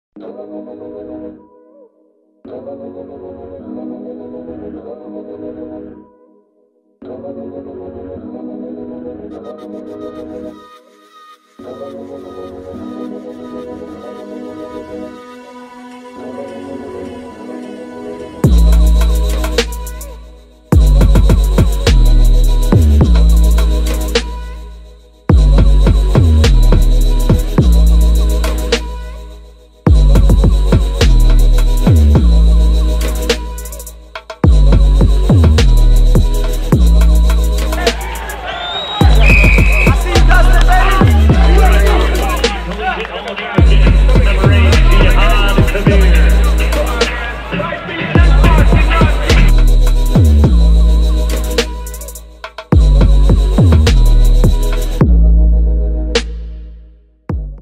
No team has more fun | USA Wins BRonze
No team has more fun. 🇺🇸️

The Next Generation of ballers have a swag that's unmatched. They are young, confident, disciplined and love the game 😎

USA Rugby wins the bronze, and Dmontae Noble and Jihad Khabir let us all know they had fun doing it 🎥🔥🥉